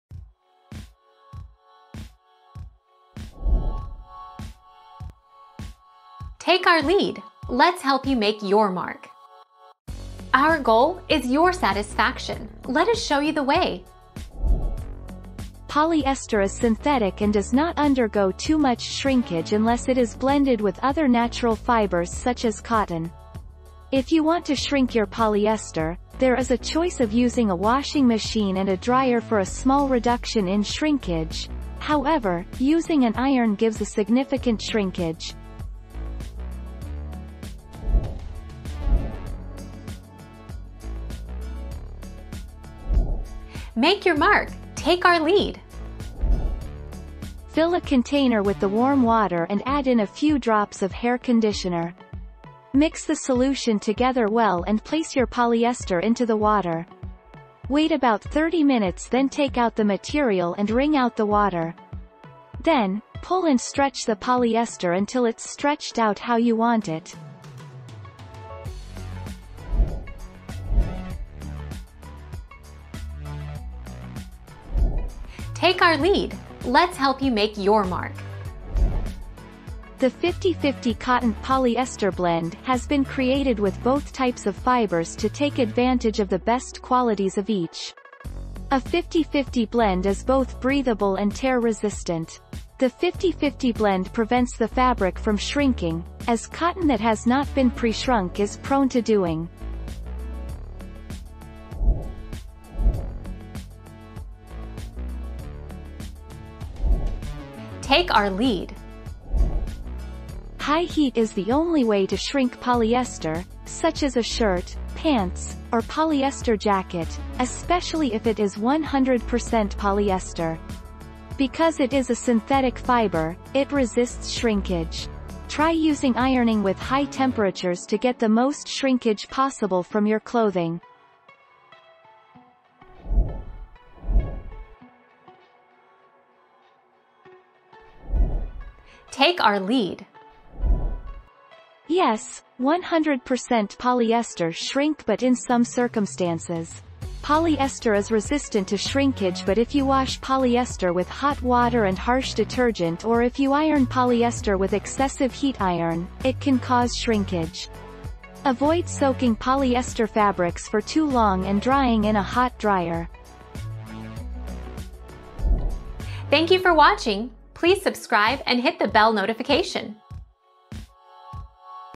Can you shrink polyester?
00:00 - Can you shrink polyester?
00:39 - How do I permanently stretch polyester?
01:11 - Does 50 percent cotton 50 percent polyester shrink?
01:43 - Is there a way to shrink 100 polyester?
02:14 - Will 100 percent polyester shrink in the dryer?

Laura S. Harris (2021, March 28.) Can you shrink polyester?
    AskAbout.video/articles/Can-you-shrink-polyester-202764